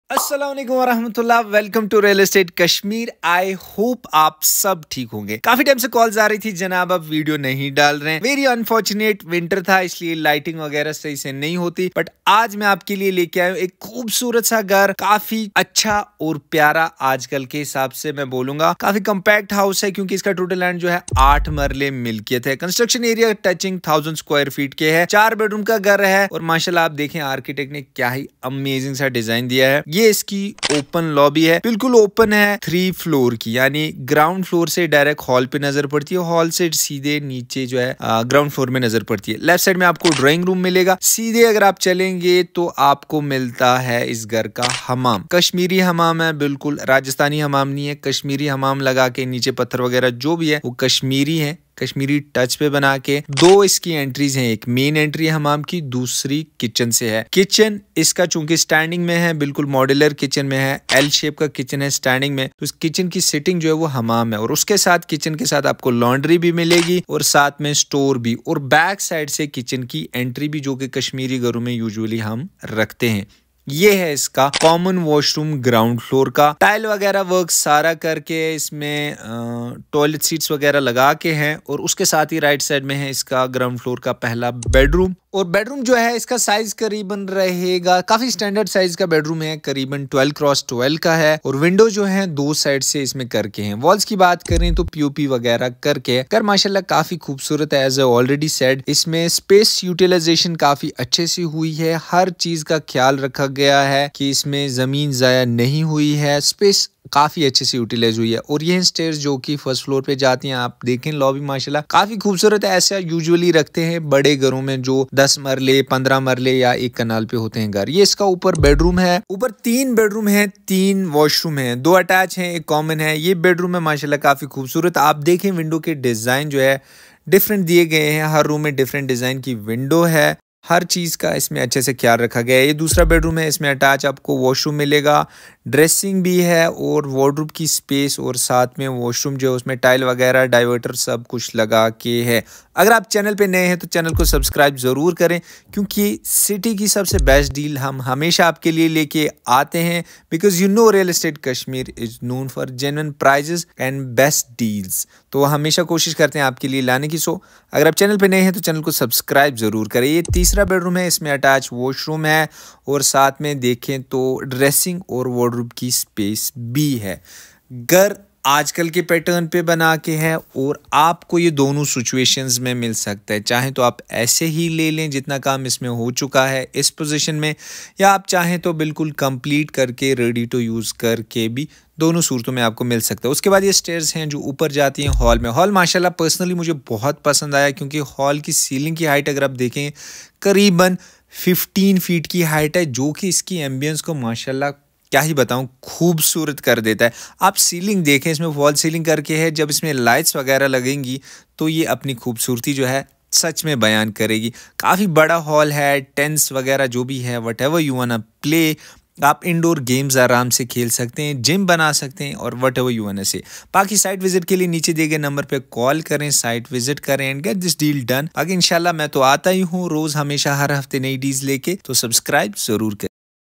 Small budget Luxury house sold in 3 days only | Realestatekashmir | sheikh Asif
Beautiful house on sale

Land :8marlas milkiyat
300mtrs from National highway

House details🎉🏠
beautiful house

🏡 Ground floor : Modular kitchen,store and Laundry hamam ,Drawing-room ,common washroom, and Bedroom

First floor: 3 beautiful bedrooms with 3washrooms

2ndfloor: big functional hall

 Completely modern Design house

Only serious clients can call or whatsapp
Call in office hours only (10am-6pm)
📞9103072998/

Address:  umerabad, Hmt

Note: Don’t call for details only if you are serious to buy visit office or call on number for showing

Price : 1.15cr